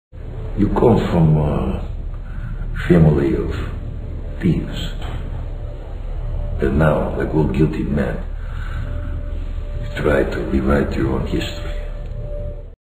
Interrogation Of The Fuzziest!
Progression on a 2D Animated short I am doing for my demo reel, and fun!